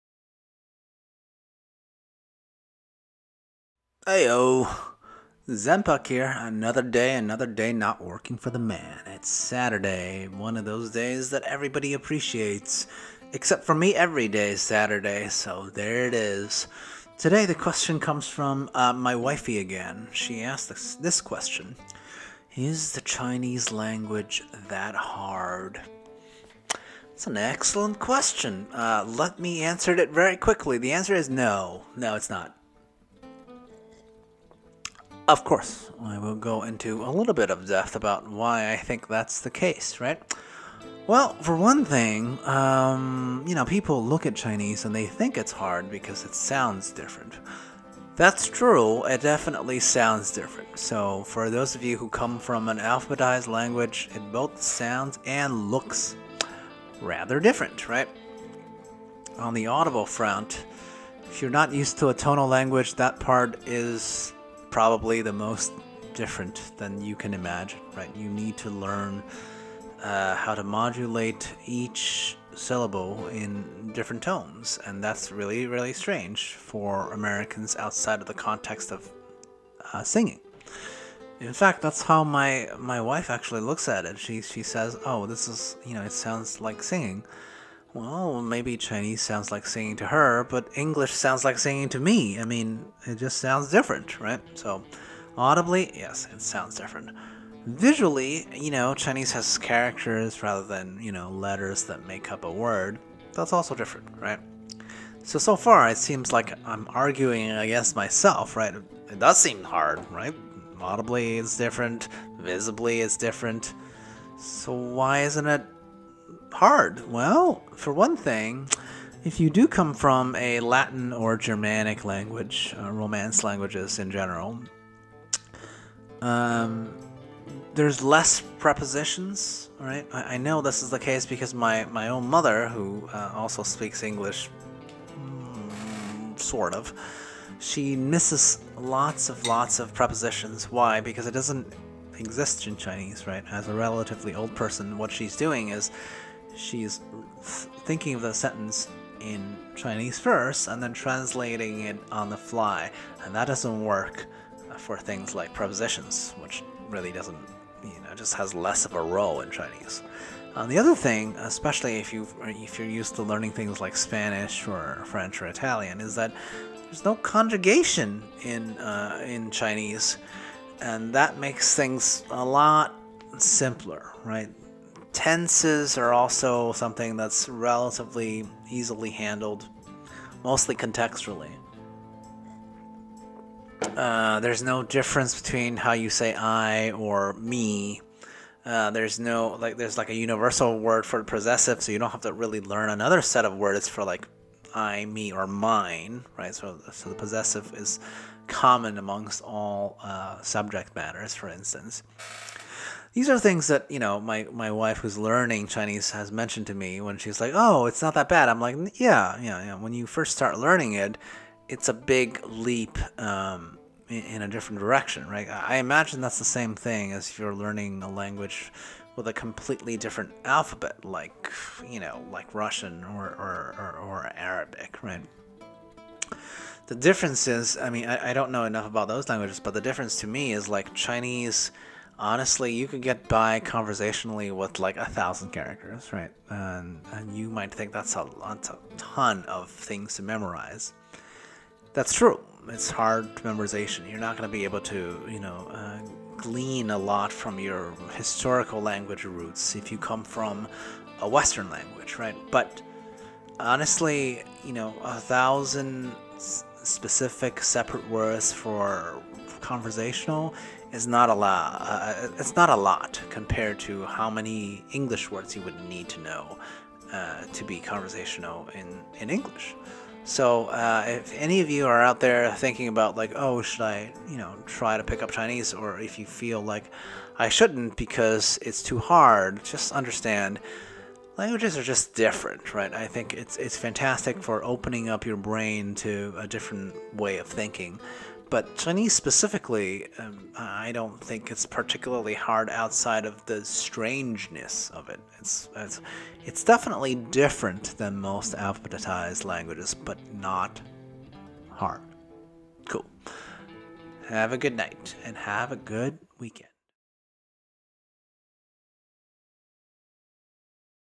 Is Chinese really that hard? Questions from Friends
just for schnitz and giggles

This is a series based on questions I've gotten from... well you guys, my new friends. So far I can speak on personal finance, China, NYC, Bay Area, the tech world, etc. Those are the things that I know but hey, I'm open to suggestions, ask me something